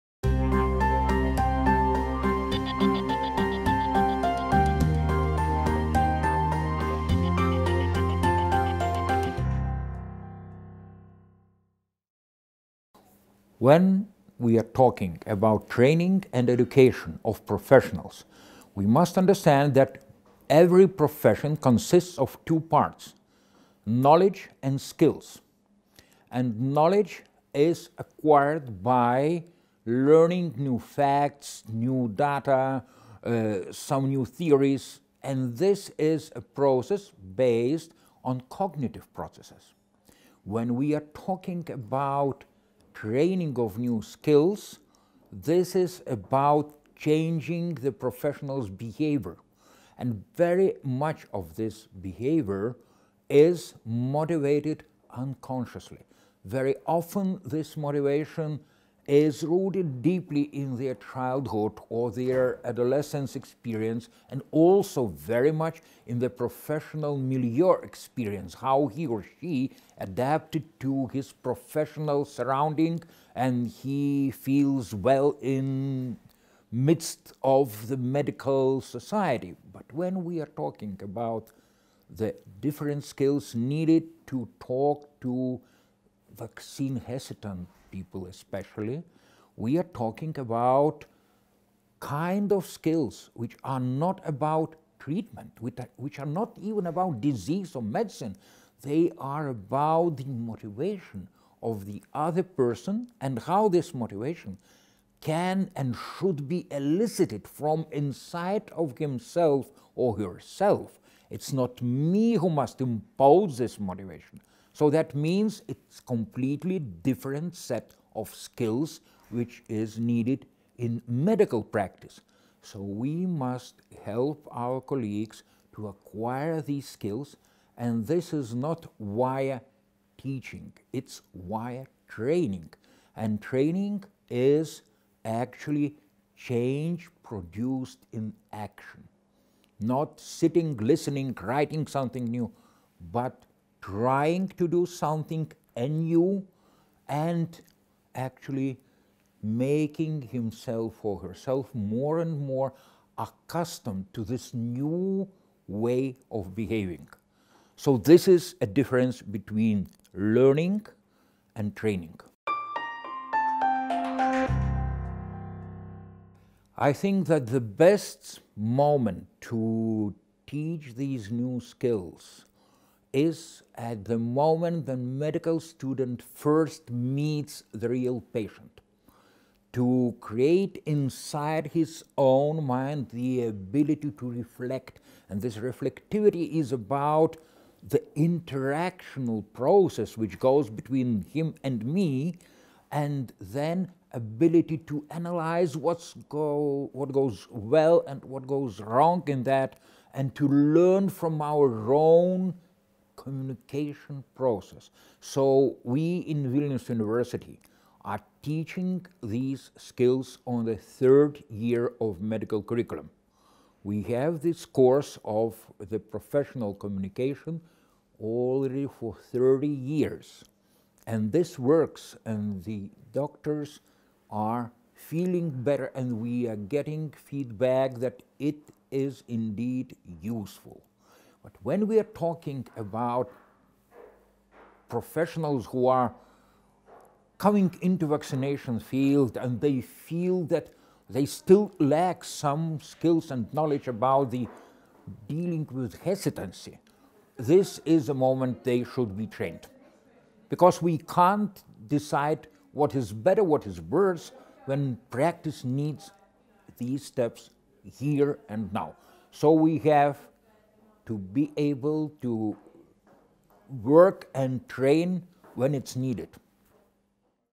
Vaccine communication: training and educating health professionals
Eugenijus Laurinaitis, University of Vilnius, discusses how health professionals can be given the skills they need to engage with people who have questions about immunisation